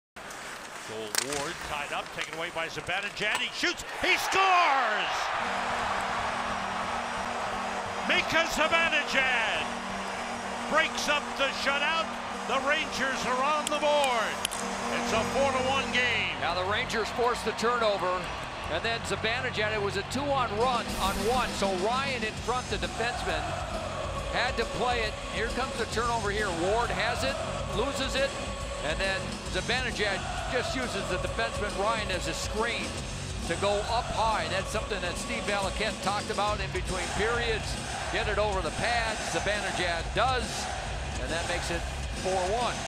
Mika Zibanejad Goal vs SJS October 23, 2017
Mika Zibanejad 3rd goal of 2017-18 nhl season. Assisted by Pavel Buchnevich